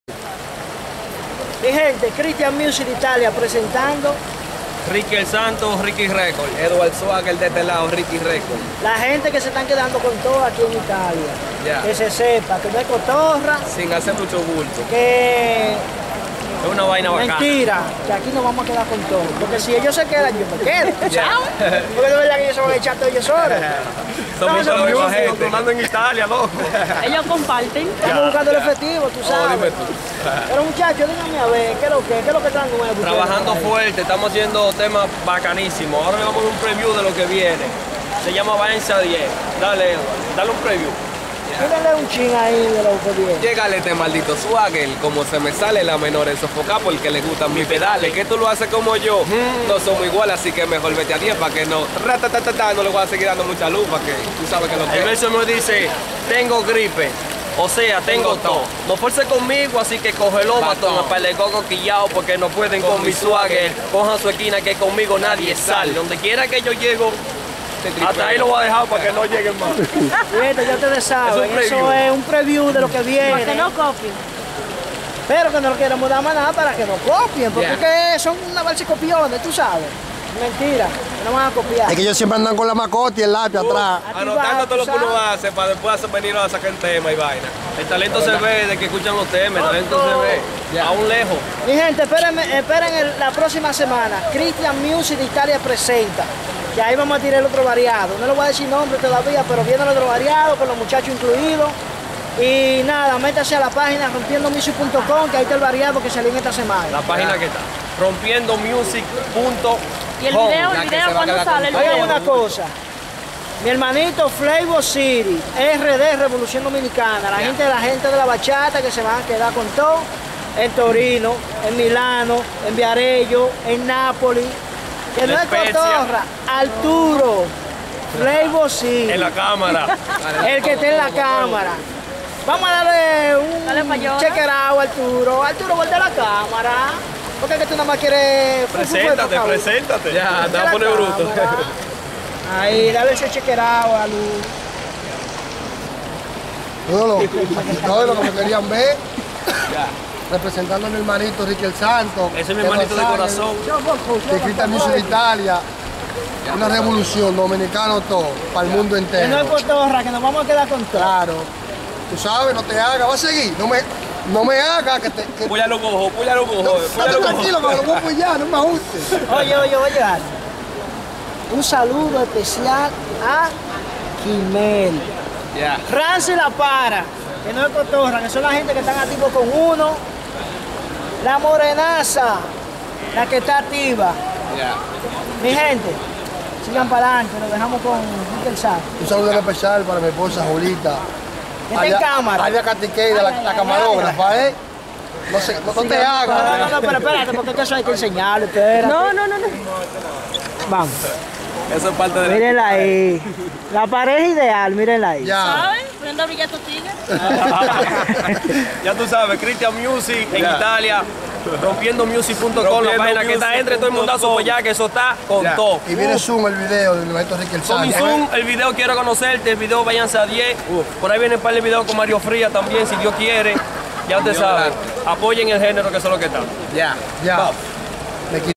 christian music the italia en chercha con la gente bacana de la spezia
esto fue una pequena entrevista a ricky el santo, eduar el swusggar y fleivo city la gente que se estan quedando con to y controlando toda europa